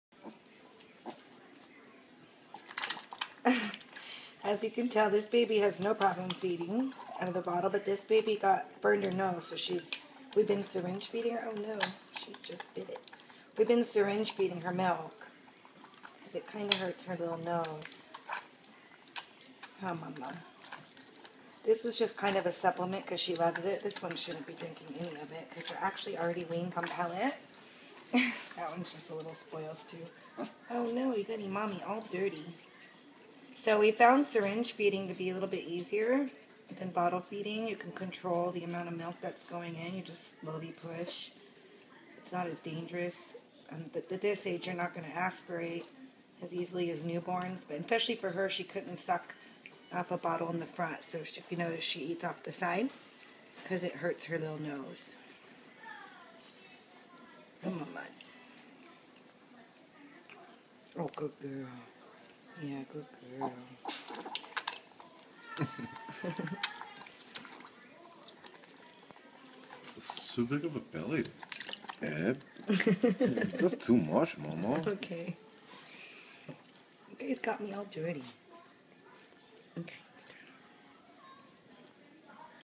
Mini Pigs Bottle Fed with Syringe
Meet Piper, a micro teacup piglet, who is being syringe fed instead it'd bottle fed due to an injury on her nose. We prefer it to bottle feeding as we can control the amount of milk the mini piglet is eating so they do not aspirate. Bottle feeding piglets, especially newborns can be dangerous. Mini pigs are not under 20 pounds full grown. Mini pigs are short and dense and most will weight 50-100 pounds full grown